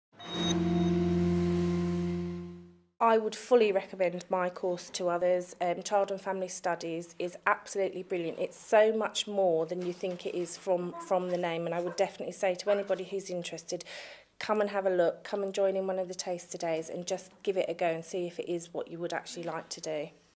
Would you recommend Child and Family Studies to others?
Find out why Cinzia Proctor would recommend Child and Family Studies to others.

To find out more about the Foundation Degree in Child and Family Studies please